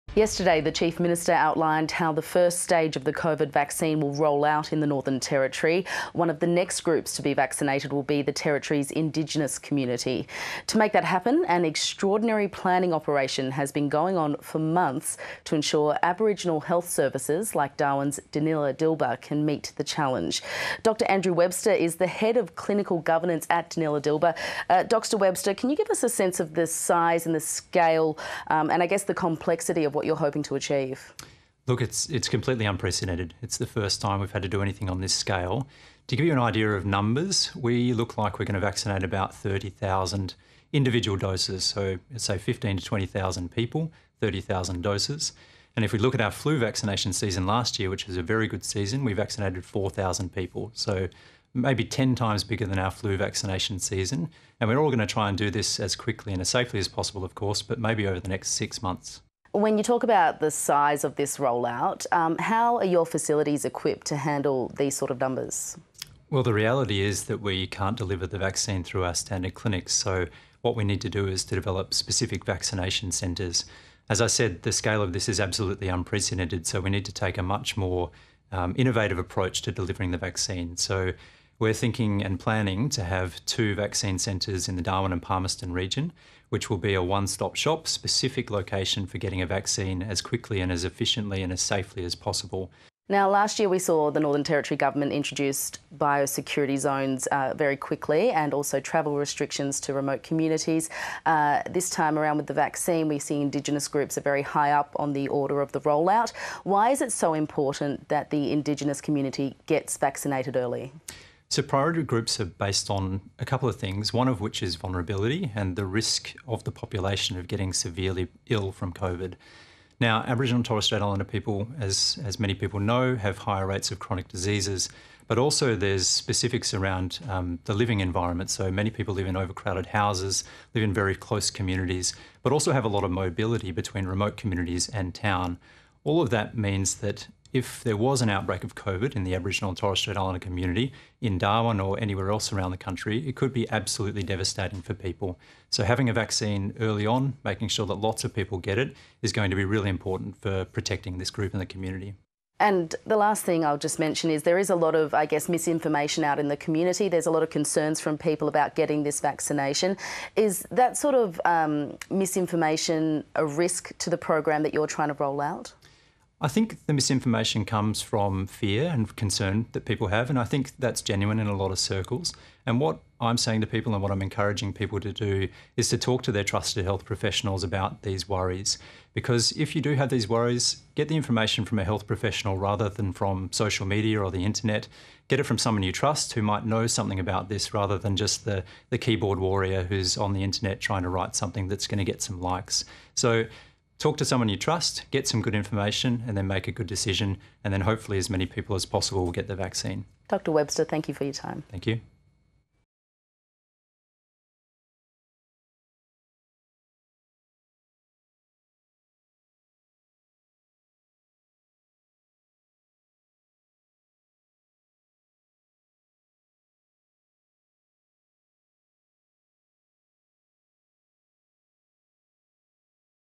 Dr Andrew Webster talks to Amy Culpitt about vaccinating Indigenous people in the NT | ABC News
As COVID-19 vaccines begin to roll out around the country one of the first groups to be vaccinated in the Northern Territory will be the Indigenous community.

An extraordinary planning operation has been going on for months to ensure Aboriginal health services like Darwin's Danila Dilba can meet the challenge.

The head of clinical governance at Danila Dilba discussed the plan with Amy Culpitt.